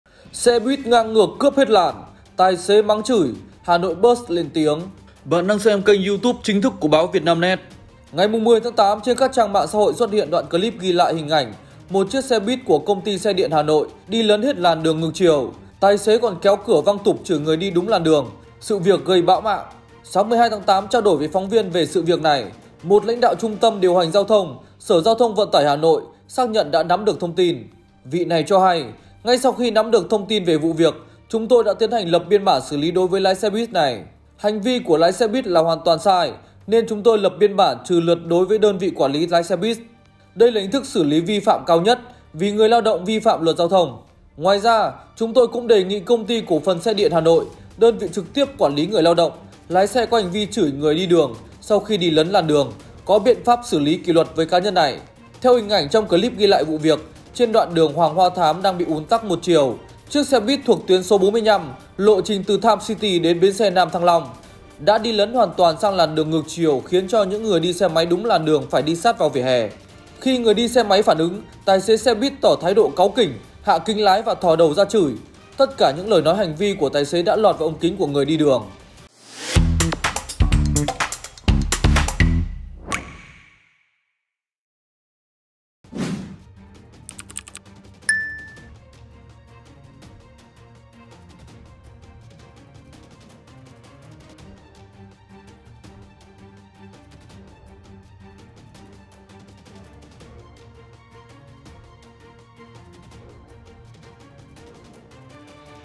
Xe bus ngang ngược "cướp" hết làn, tài xế mắng chửi người đi đường | Vietnamnet
Xe bus số 45 đi cướp hết làn, tài xế còn kéo cửa chửi mắng người đi đường. Công ty quản lý xe này đã bị đơn vị quản lý xe bus Hà Nội lập biên bản xử phạt với hình thức cao nhất là trừ hết lượt chạy.